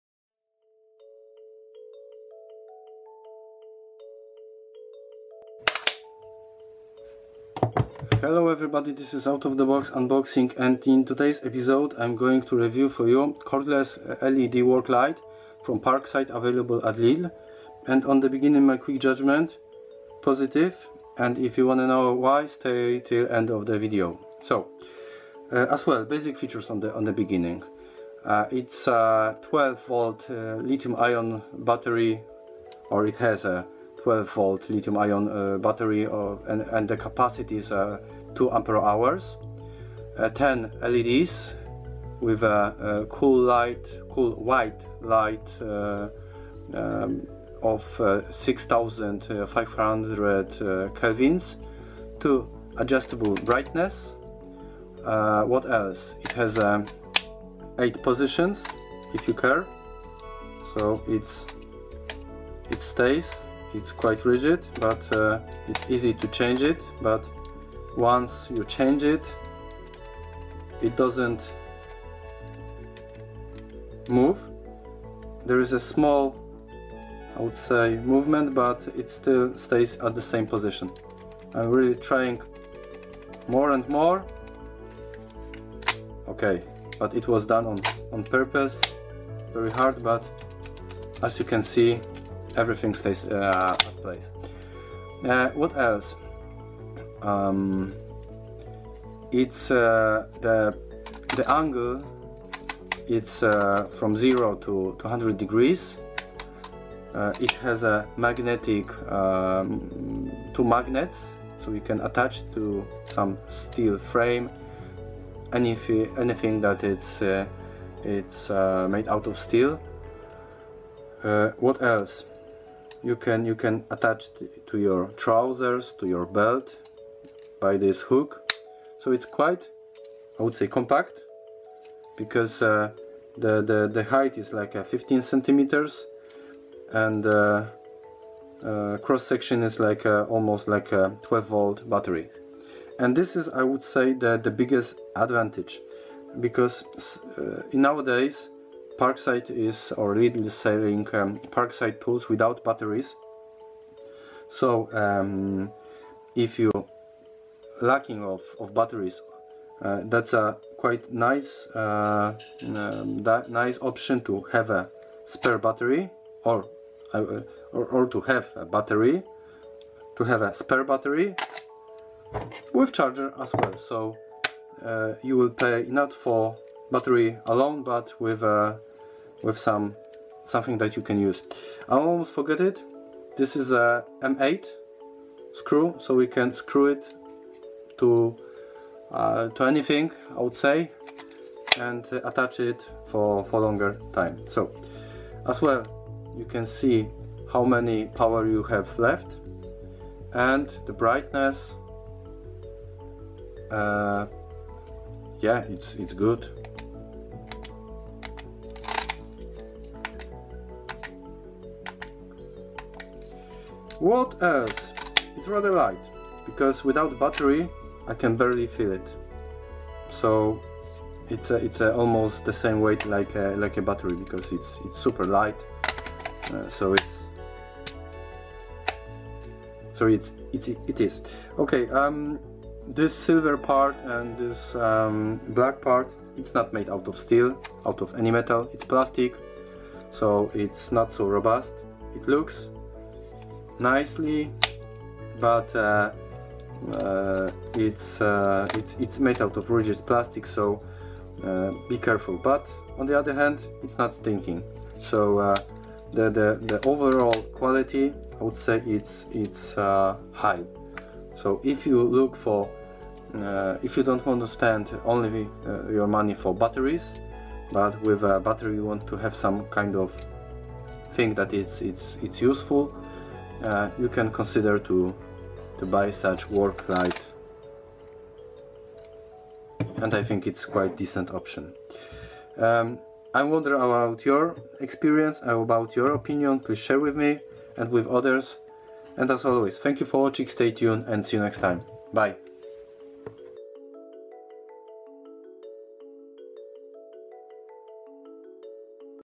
Parkside Cordless LED Work Light PLLA 12 B2 REVIEW / TEST (Lidl 10 LED 2.0Ah 6500K)
IAN: 323150

Features
Cordless LED Work Light PLLA 12 B2
Powerful 12V Li-Ion battery (2.0Ah)
10 LEDs with a cool white work light (6500K)
2 adjustable brightness levels
Luminous head tiltable to 8 positions
Robust housing with magnetic attachment, belt clip and tripod thread (¼')
3 stage battery charge level indicator
60-minute fast charger with automatic switch-off